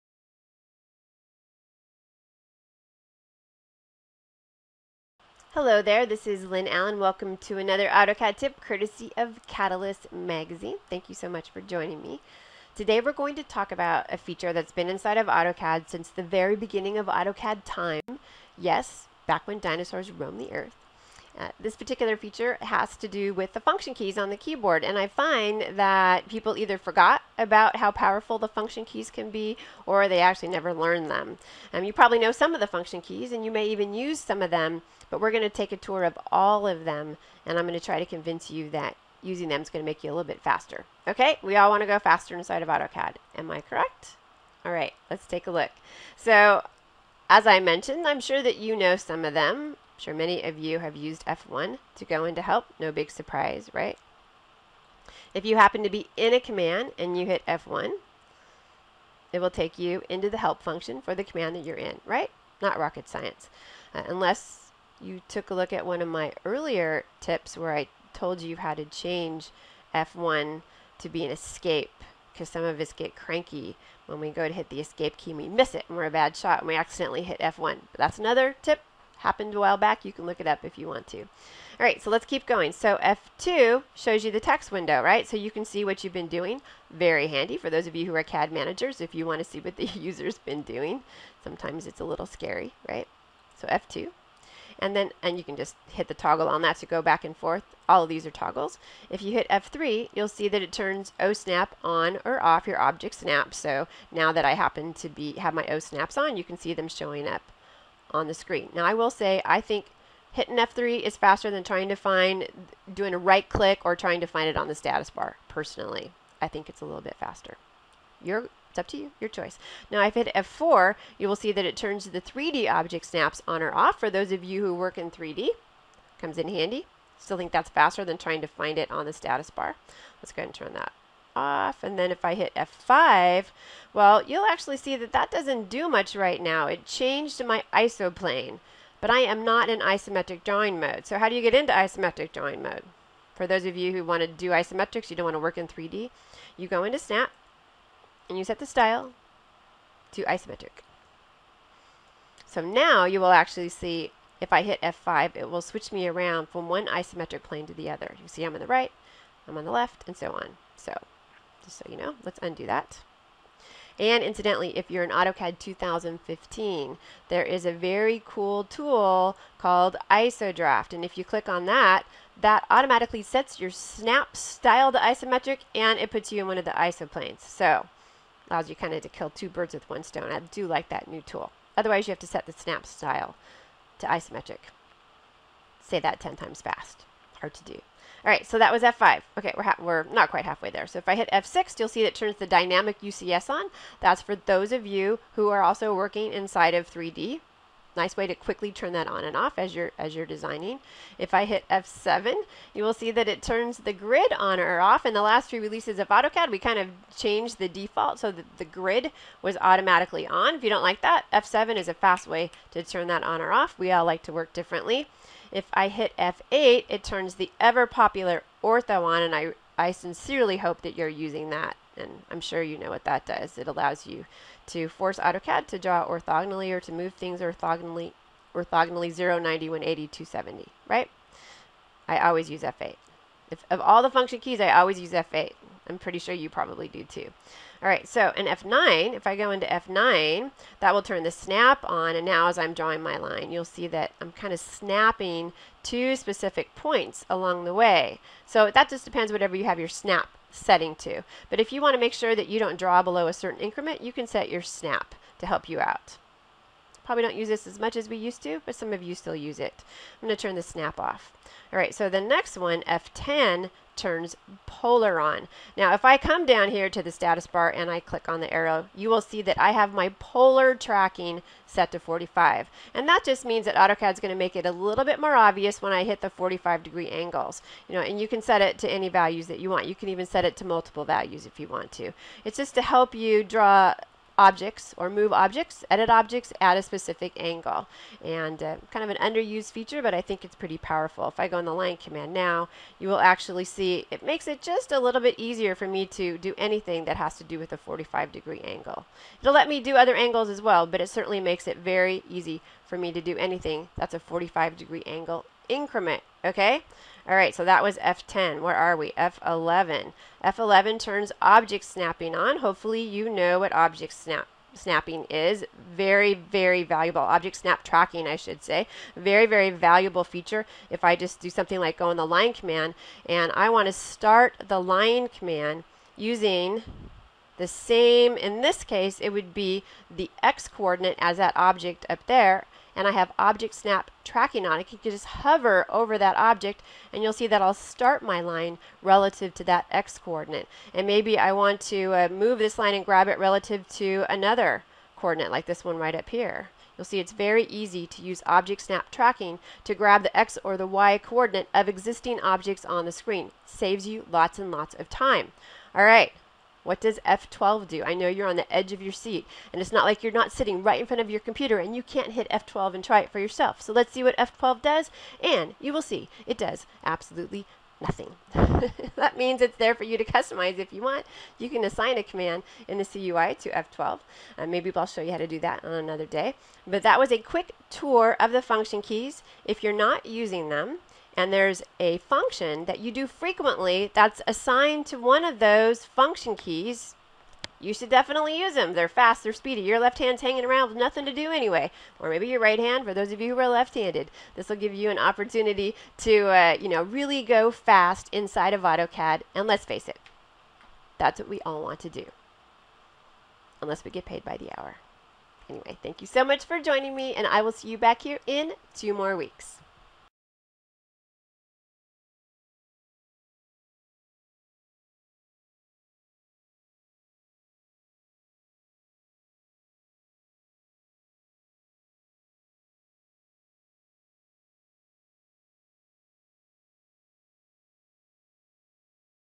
Reviving AutoCAD's Forgotten Function Keys (Lynn Allen/Cadalyst Magazine)
Maybe you’ve forgotten about them (or never learned to use them), but using your function keys to toggle important settings inside AutoCAD can really speed up your design process. With this video tip from Cadalyst and Lynn Allen, you’ll learn how to take advantage of all that the function keys have to offer.